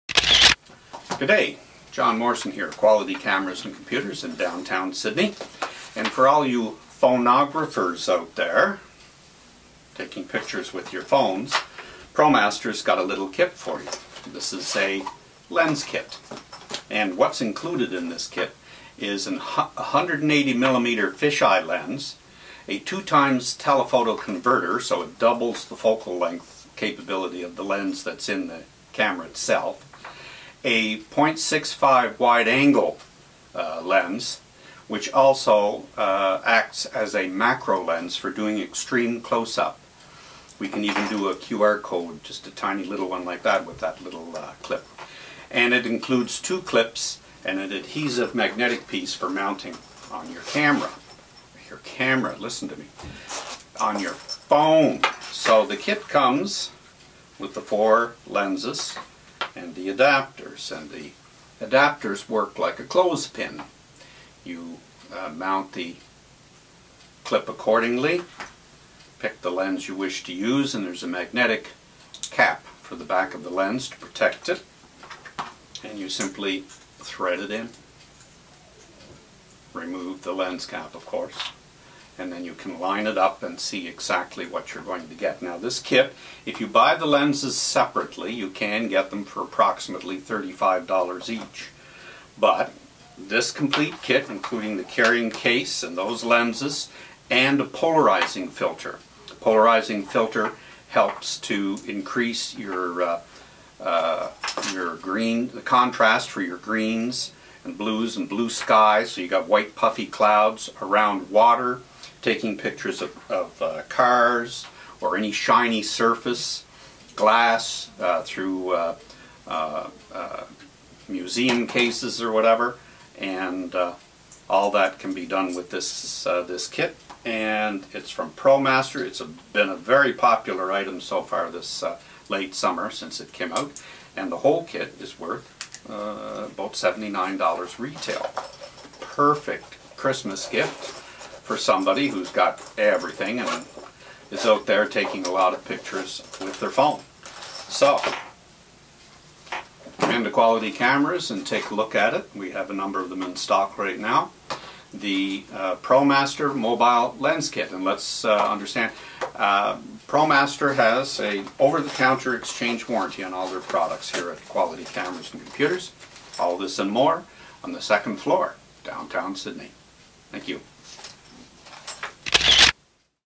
PHONE LENSES
Take your phone photography to the next level.  This lens kit gives you all the versatility to be creative with your phone.  Wide angle, fisheye, macro lenses and more.  Check it out.